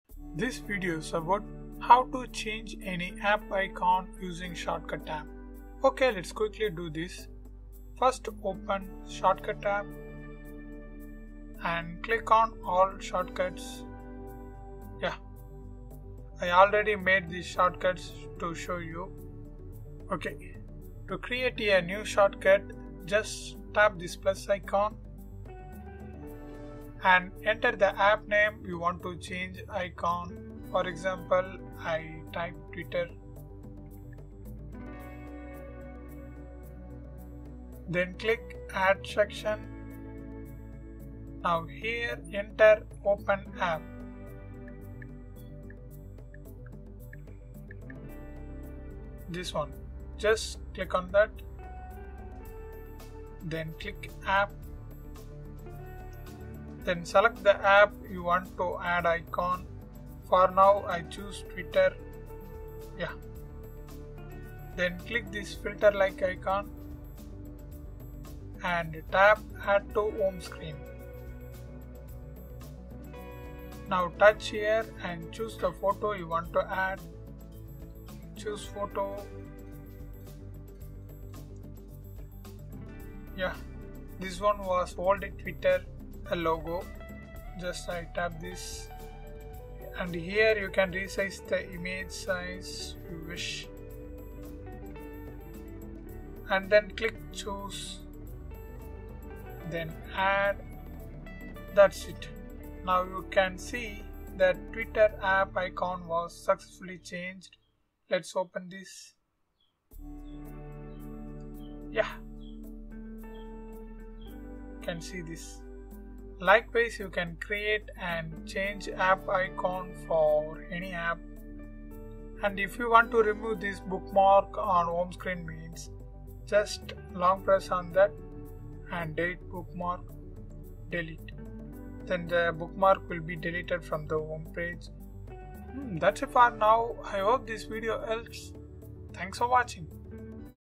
How to Use Shortcuts to Change App Icons (2025)
How to Use Shortcuts to Change App Icons in 2025? In this video i will showed you about how to change or customize your iphone or ipad app icons by using shortcuts app. Actually you can easily change and customise your apps by using shortcut app with few steps. Just follow our daily doubts video above. That's it. Your are Done.